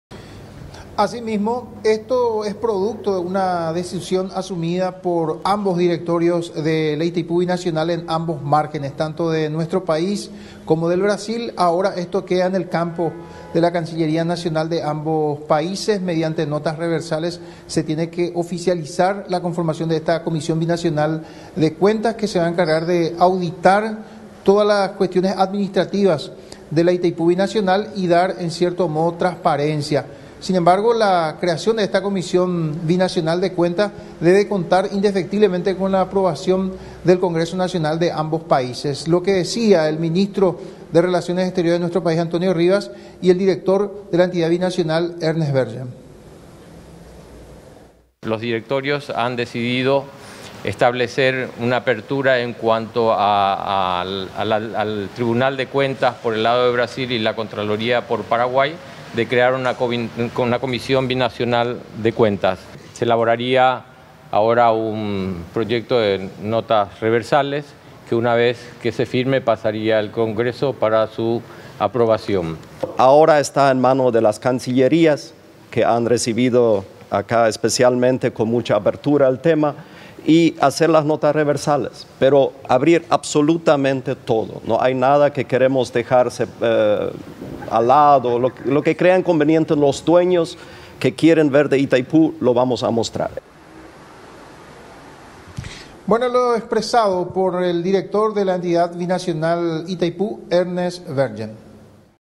Comisión Binacional para auditar Itaipú
La Itaipú Binacional podría ser auditada por las Contralorías de Paraguay y Brasil mediante la creación de una Comisión de cuentas planteada por ambos directores de la Binacional a las cancillerías.